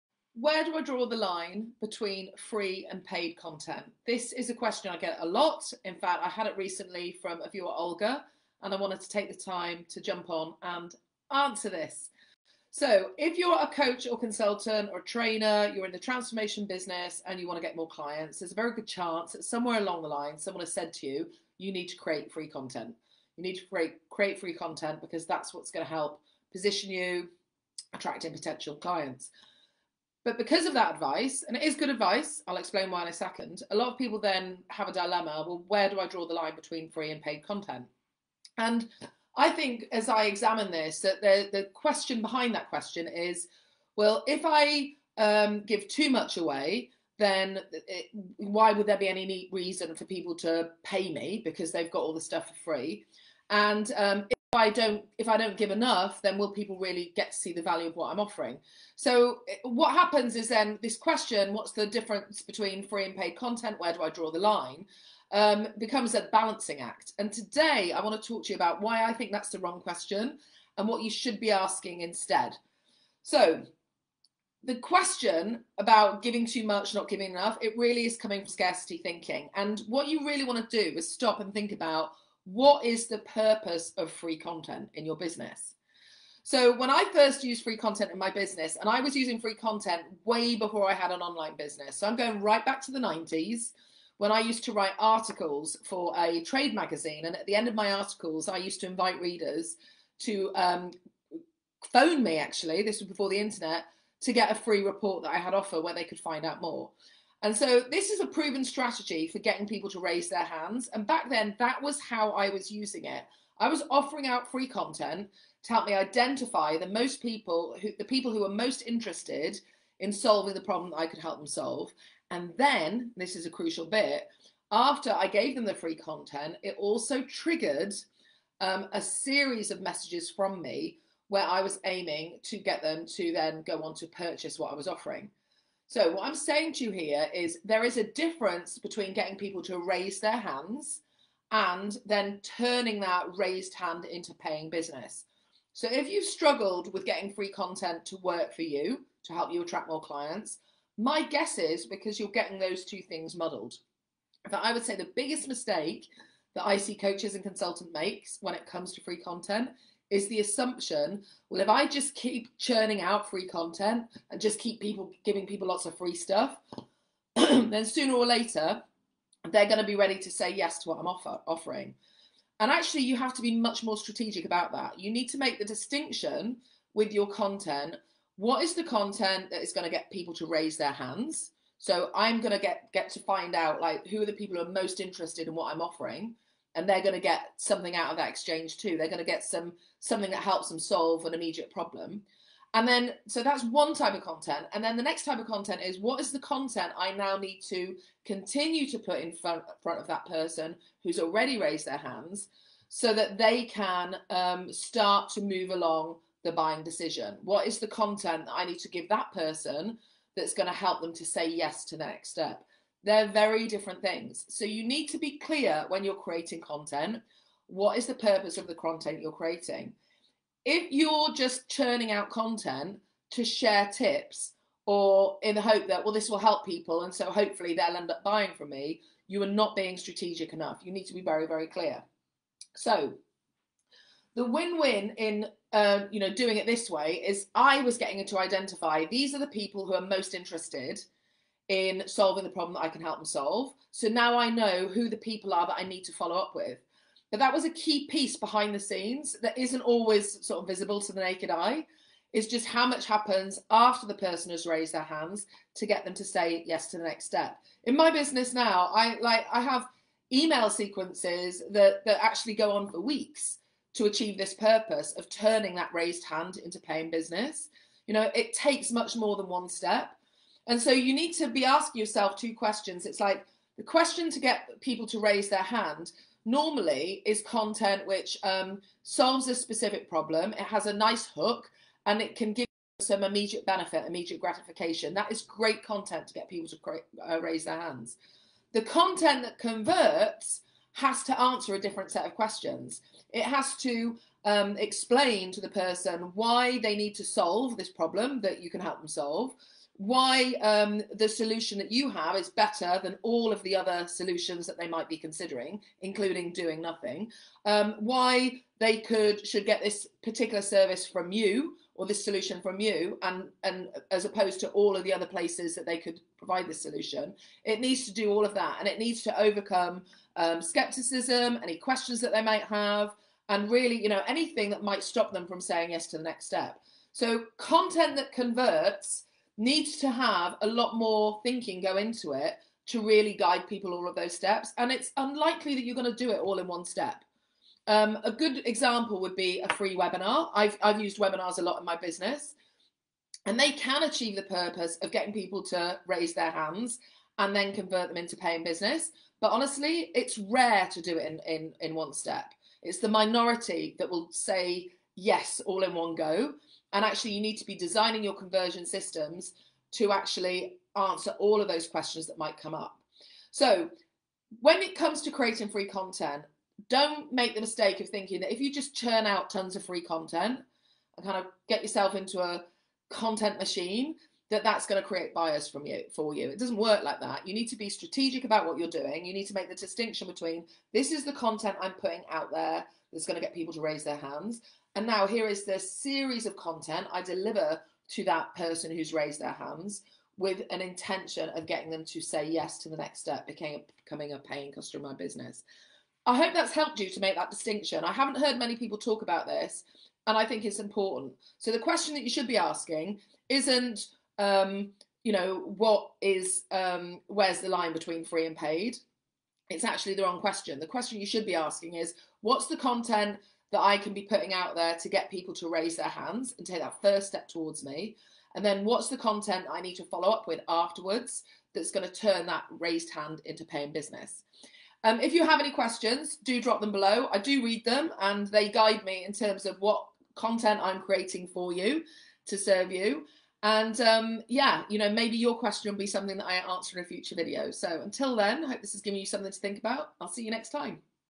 Free vs Paid Content - are you giving away too much?
Free vs Paid Content. What is the difference between free content and paid content? How to know if I’m giving away too much for free? Watch this video and find out how to differentiate free content and paid content and to know if you are giving away too much for free.

Want a plan to get more coaching clients and make more money? Download my FREE guide 'The ABC of Consistent 10k+ Months'